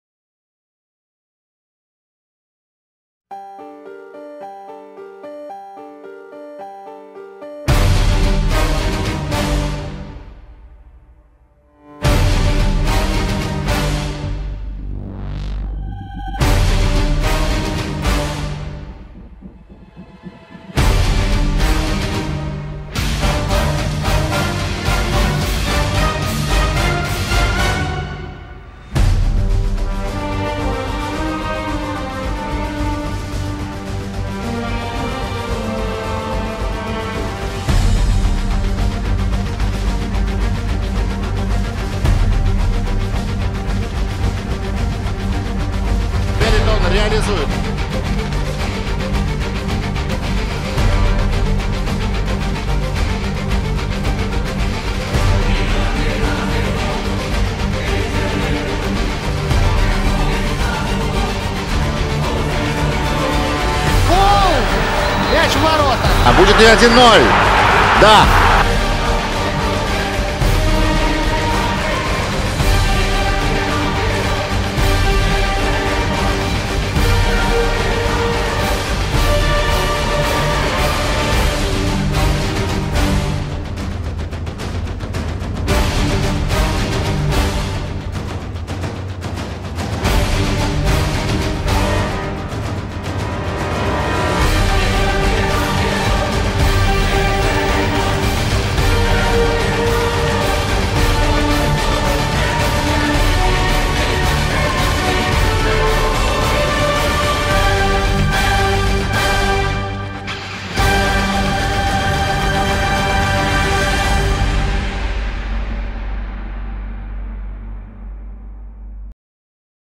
Preview Rubin-Spartak
Превью к матчу 27-тура Чемпионата России по футболу.
Program: Sony Vegas 10 and Adobe After Effect cs 4